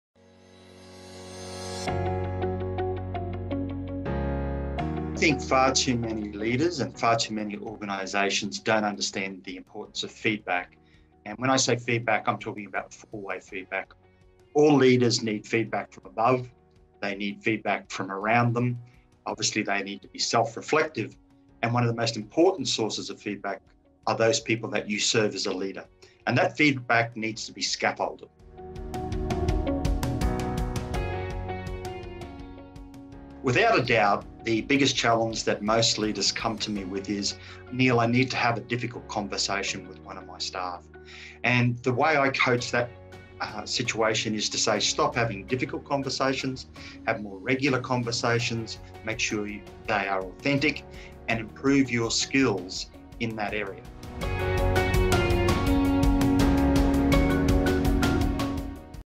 Dr Neil Carrington on the one mistake all leaders make: ACE Leadership Masterclass
Dr Neil Carrington is one of Australia's most respected leadership speakers. In a series of dynamic full-day Masterclasses around Australia (Jul–Sept 2021) for the Australian College of Educators, Dr Carrington will reveal how to lead with wisdom, empathy and authenticity.

Here, we asked him about the one mistake all leaders make.

This Masterclass will change the way you think about leadership. Don't miss out!